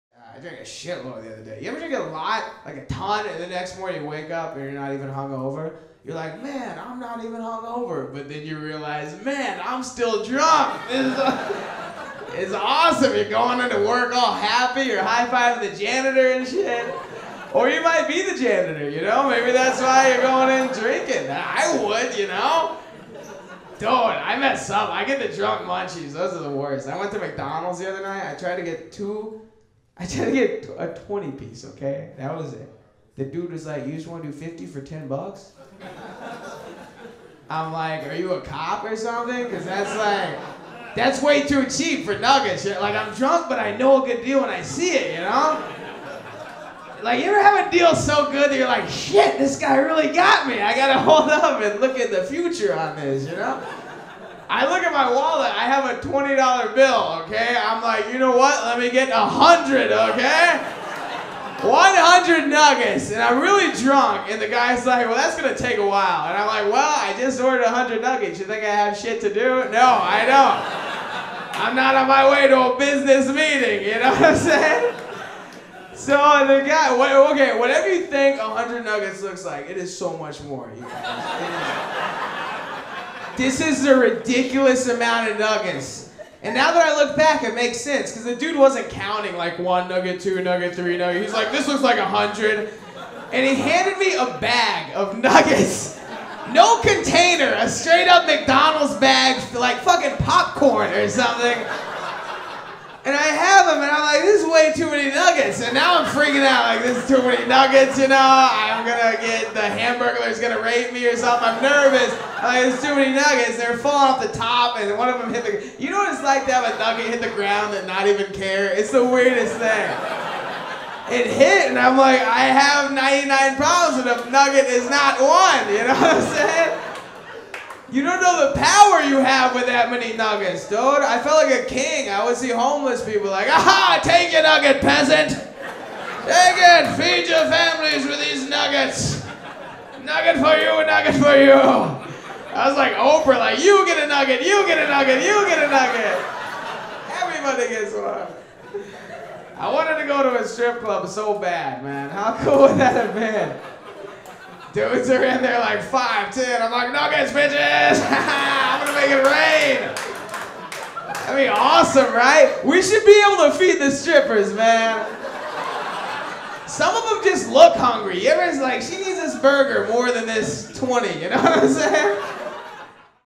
Dustin Ybarra - Going to McDonalds Drunk (Stand Up Comedy)
Dustin loves chicken nuggets.

Come see a live performance if you want to be part of the real fun!